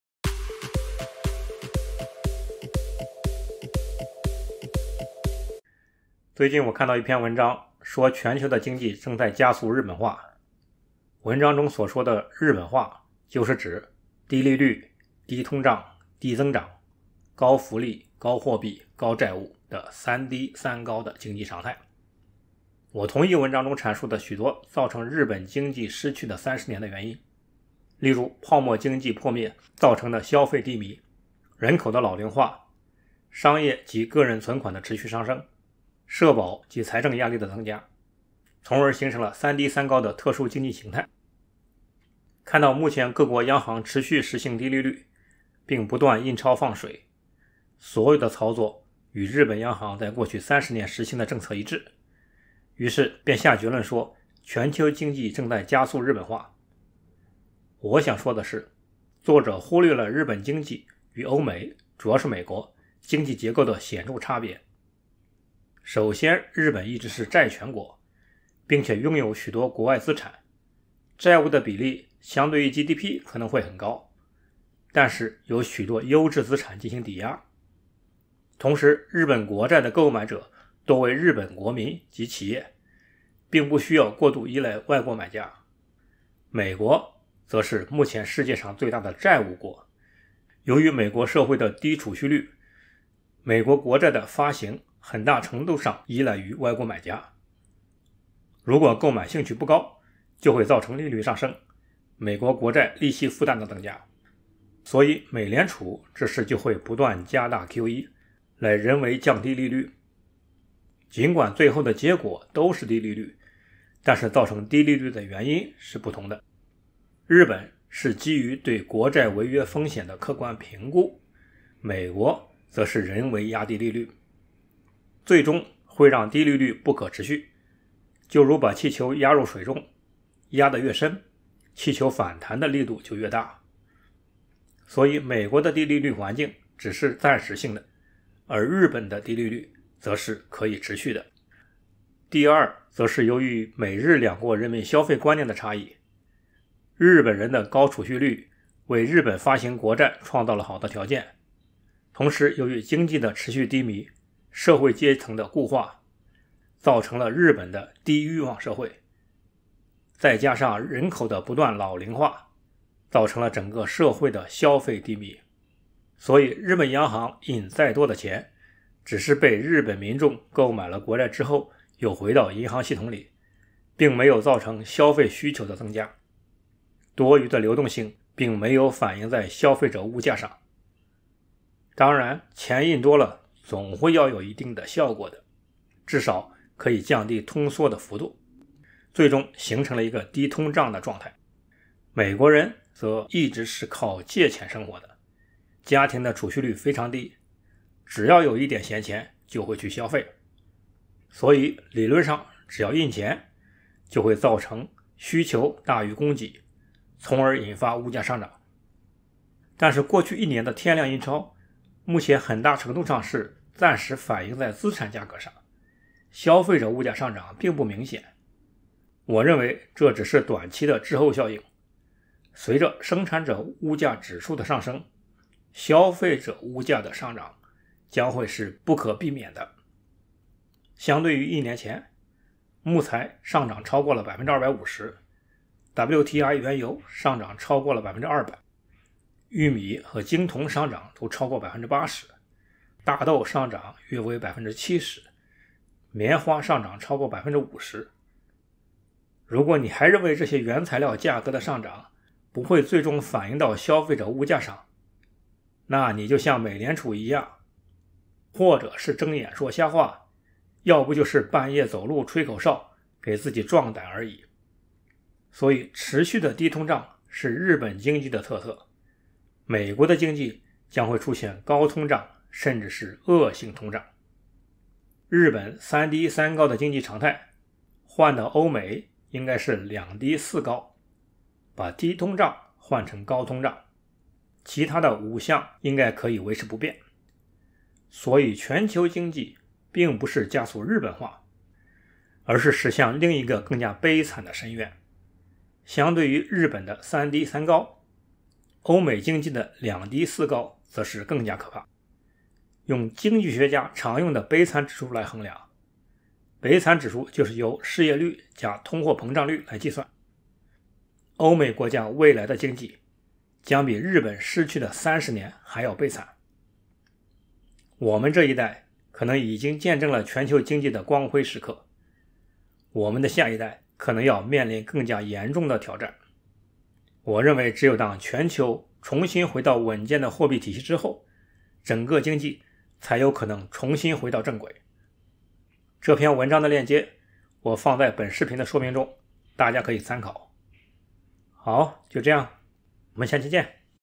EP61 - 世界經濟在加速日本化? Is the Global Economy Racing Toward Japanification?
這一期，我和大家一起解讀一篇文章，談論世界經濟日本化的觀點。我個人同意文章中大多數的觀點，但我認為歐美與日本經濟的主要差別在於高通脹的發生。

In this episode, I will interpret an article with you, discussing the viewpoint of the Japanification of the world economy. I personally agree with most of the points in the article, but I think the main difference between the economies of the West and Japan lies in the occurrence of high inflation.